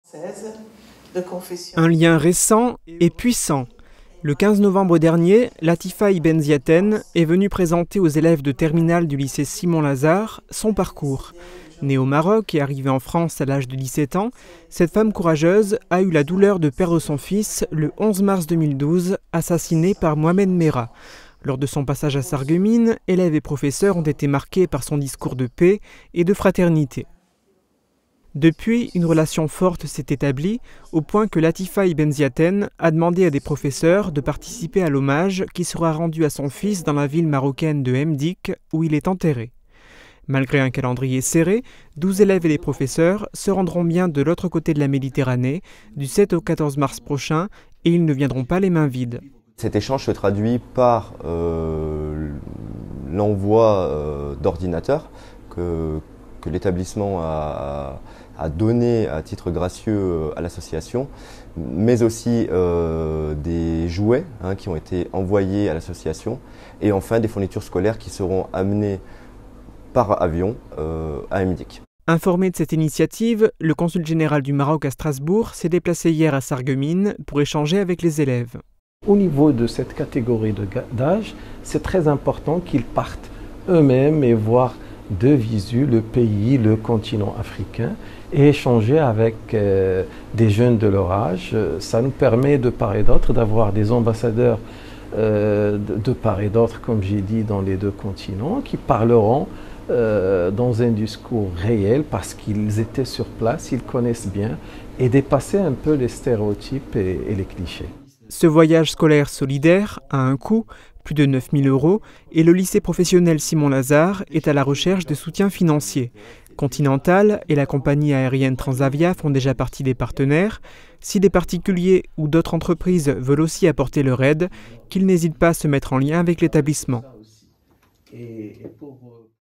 Des élèves du Lycée Simon Lazard ont reçu la visite du Consul Général du Maroc
12 élèves du Lycée professionnel Simon Lazard vont effectuer un voyage solidaire d’une semaine au Maroc. Pour témoigner de l’importance que revêt cet événement, le Consul Général du Royaume du Maroc en personne, s’est déplacé à Sarreguemines pour échanger avec eux.
Réalisation : Guillaume DUMONTEIL